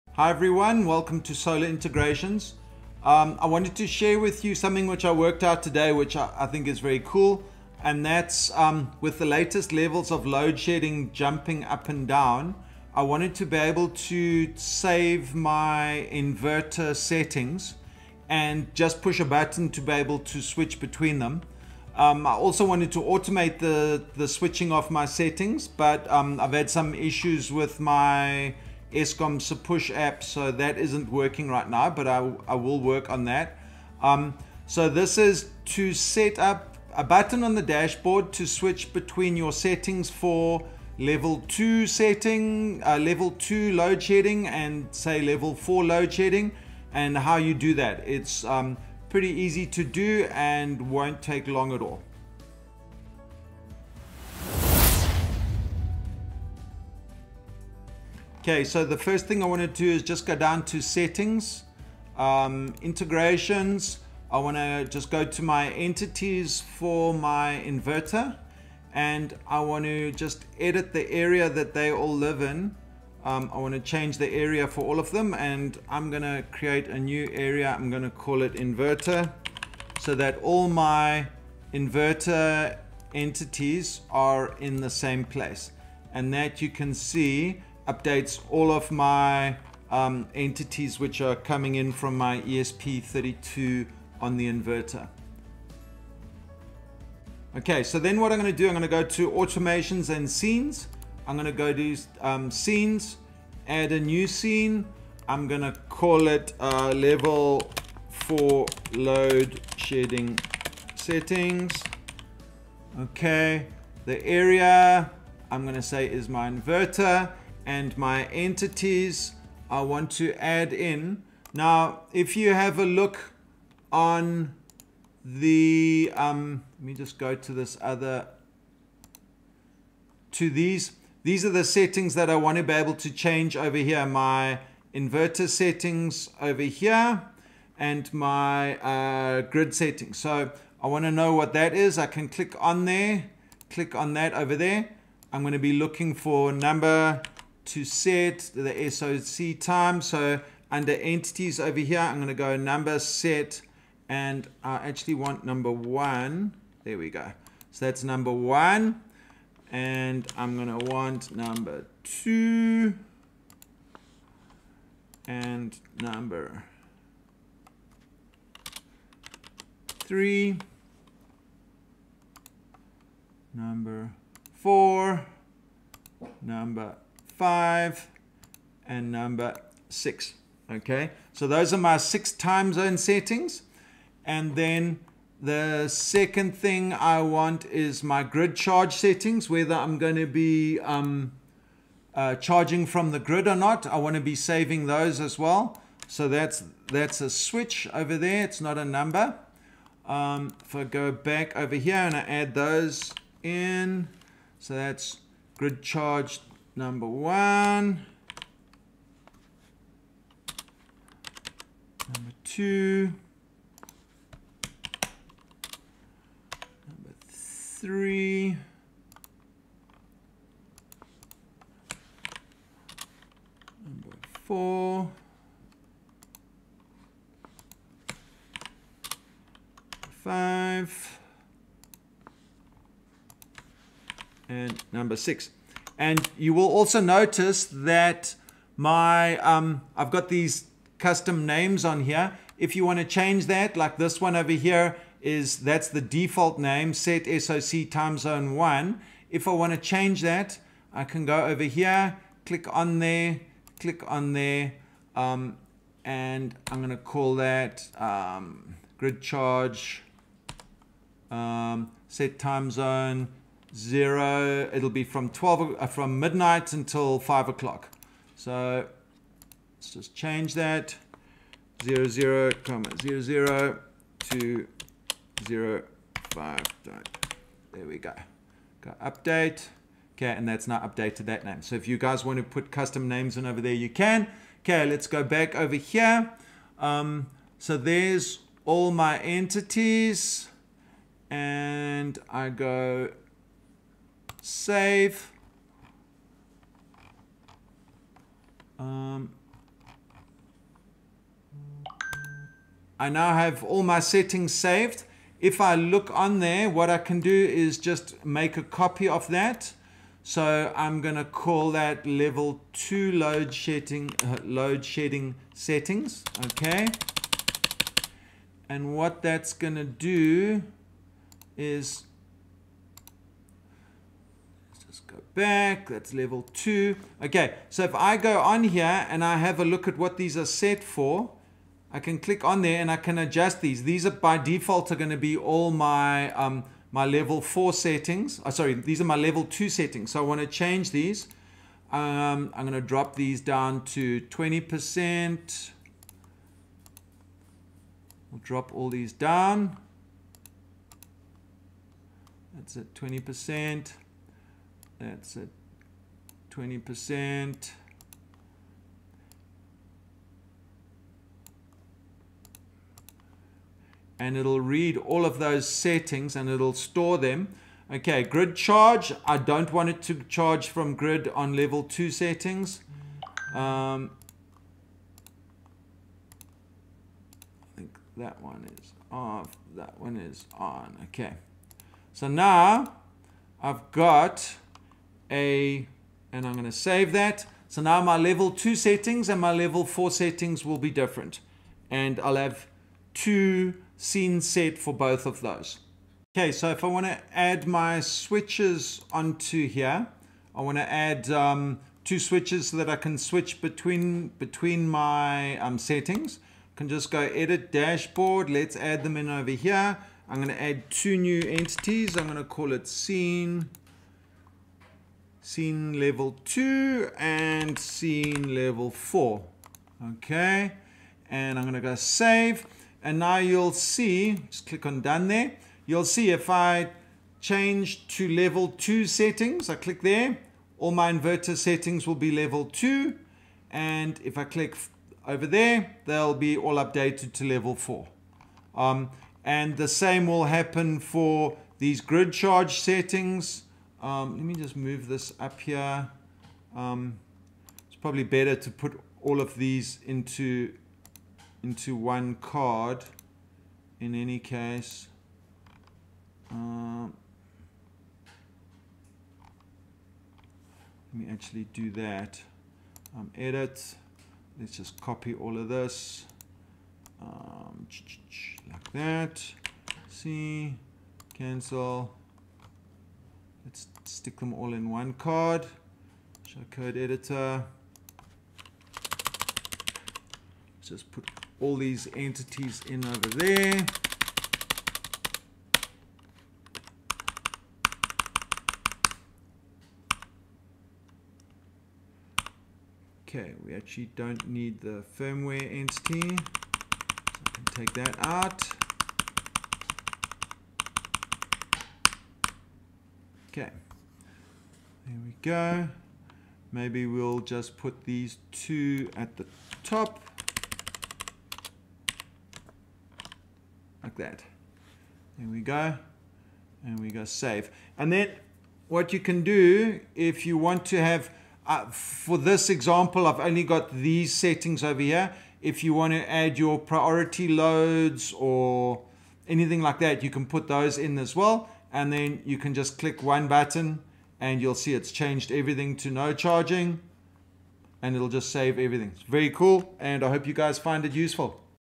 Easily Switching between Saved Inverter Settings
This video demonstrates how to set up your home assistant to enable you to switch your inverter settings by pushing one button on your dashboard.

Uses the ESP32 Integration, can be done using the Sunsynk integration as well but requires the writing of values to be enabled.

Is very handy with all the changes in Load Shedding levels recently.

I will do an automation to switch the level automatically once my EskomSePush profile get's unblacklisted.